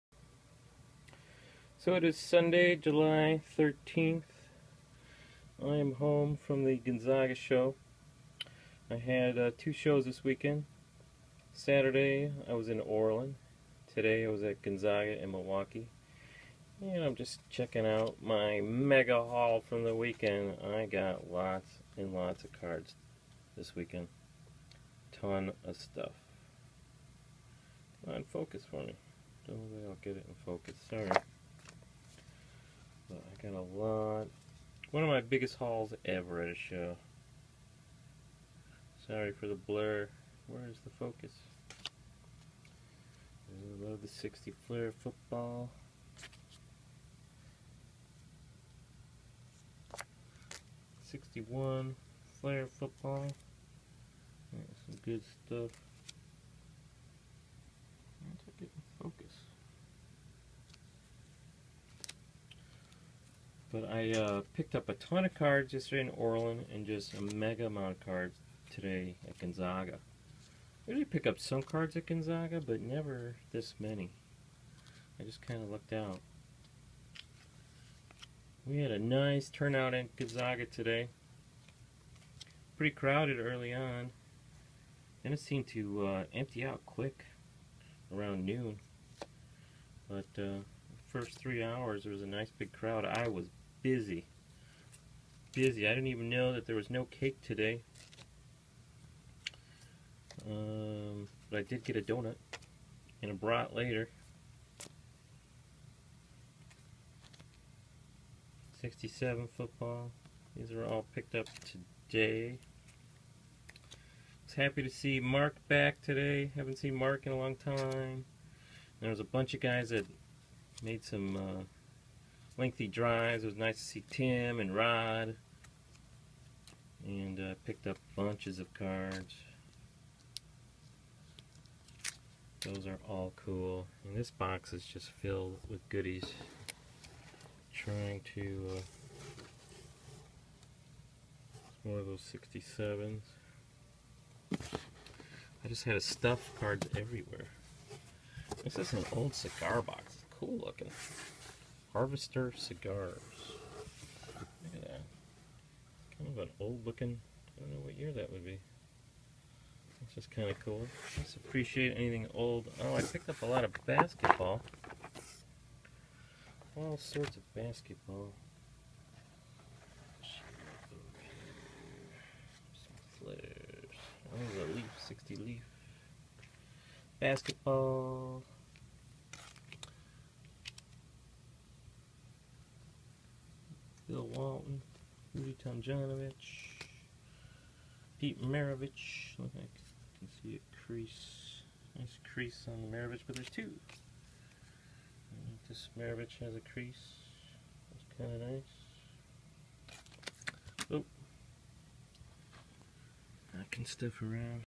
July 2014 Gonzaga Purchases 1
First of two videos of my vintage baseball card purchases at the Gonzaga Card Show in Milwaukee